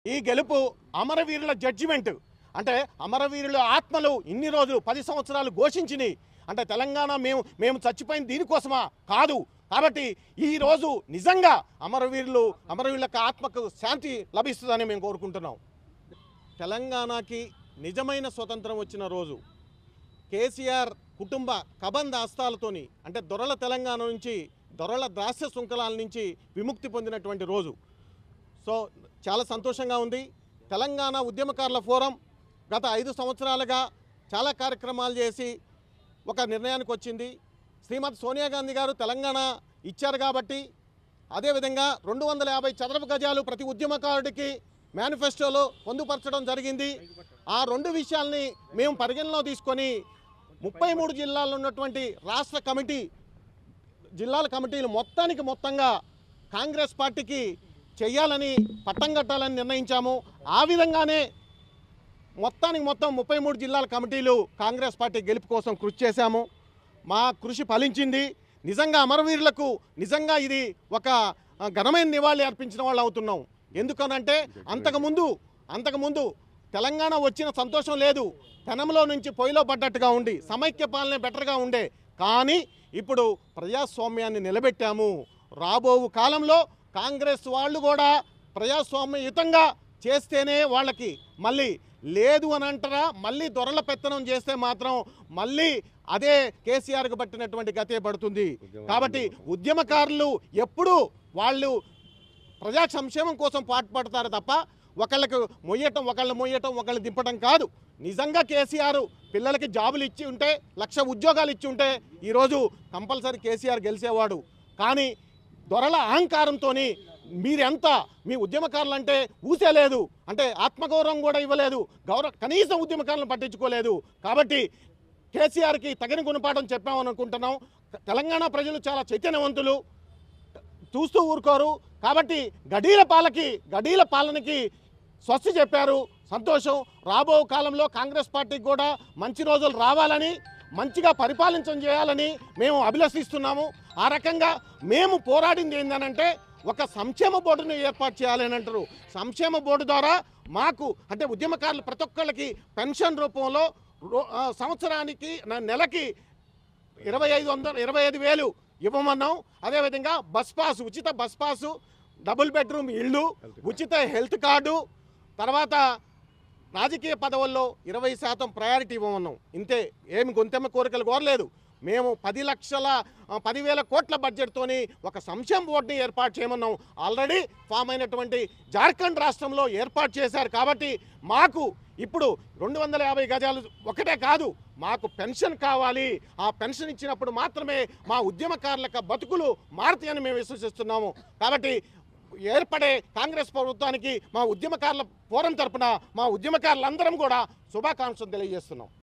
కాంగ్రెస్ విజయం పూర్తిగా అమరవీరుల గెలుపుగా కె.వికృష్ణారావు
కాంగ్రెస్ విజయం పూర్తిగా అమరవీరుల గెలుపుగా కె.వి
కృష్ణారావు